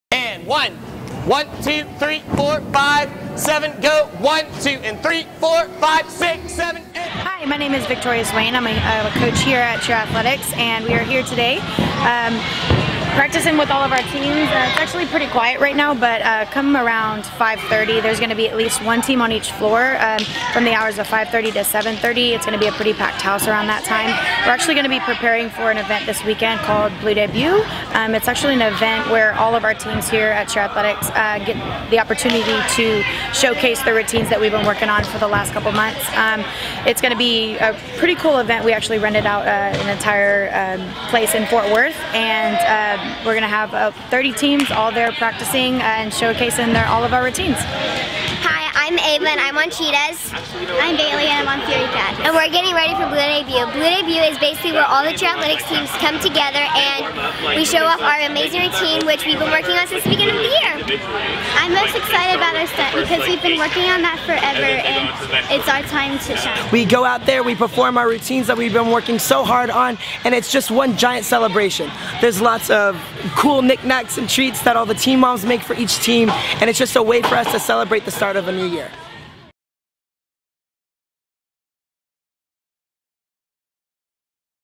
2016 Cheer Athletics Blue Debut: Coach & Athlete Interviews
The 2016 Cheer Athletics Blue Debut is tomorrow! We caught up with coaches and athletes as they prepared for the showcase.